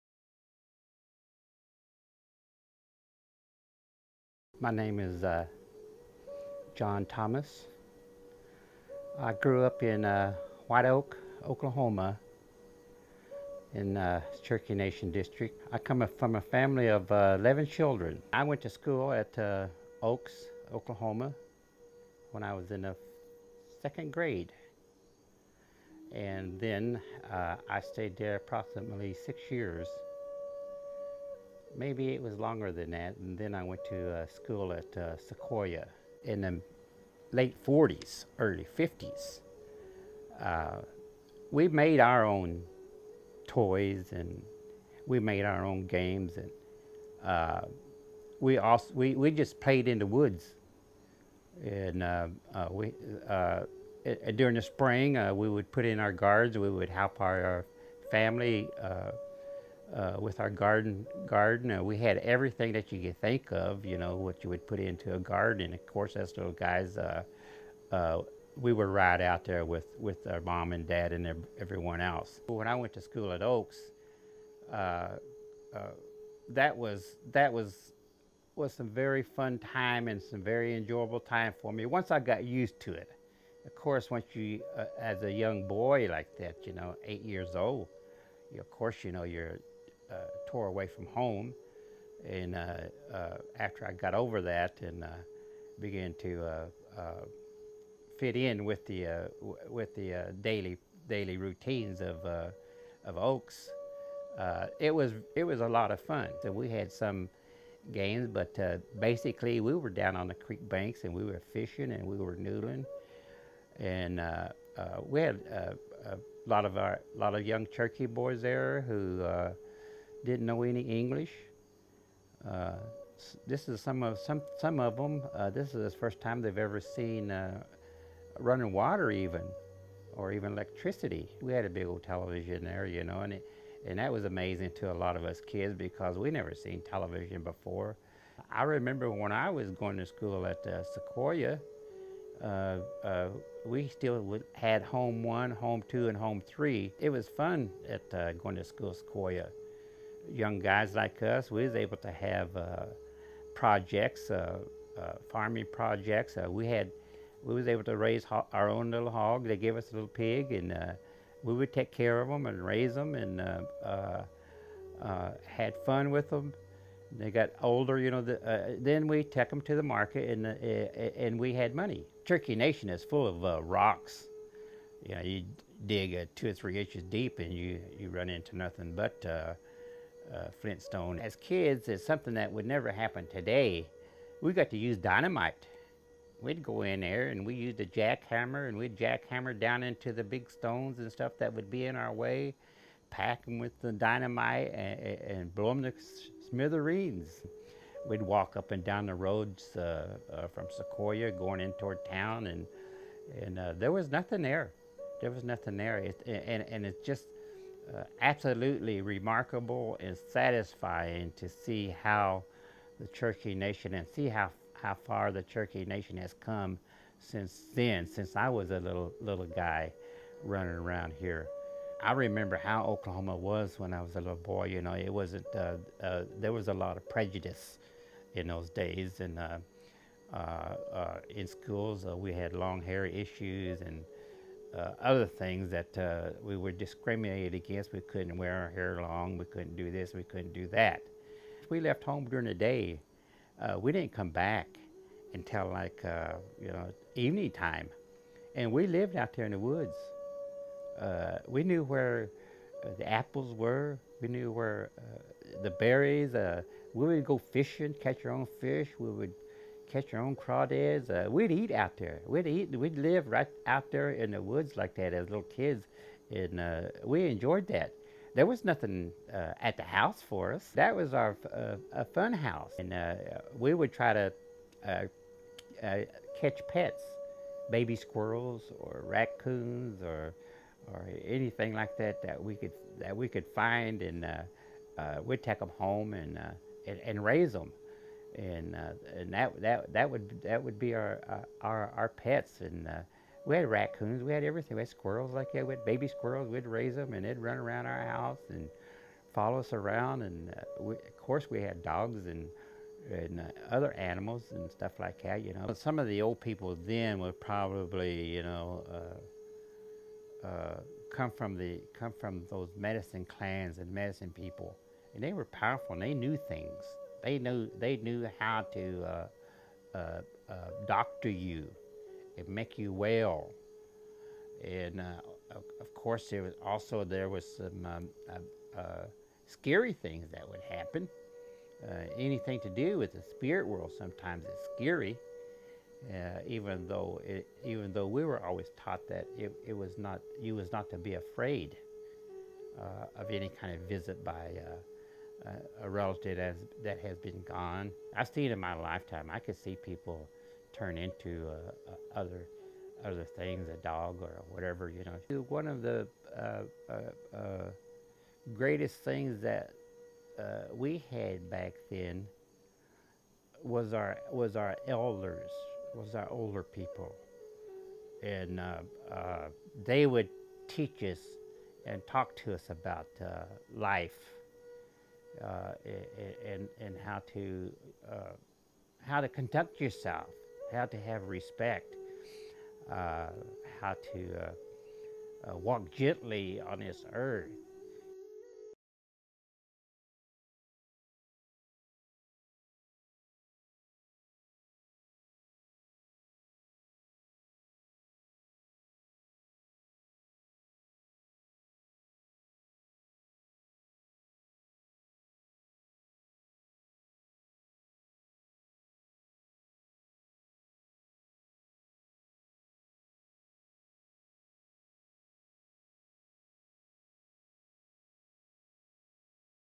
Cherokee Nation's Back Then John Thomas
Cherokee Elder John Thomas talks about growing up Cherokee back then.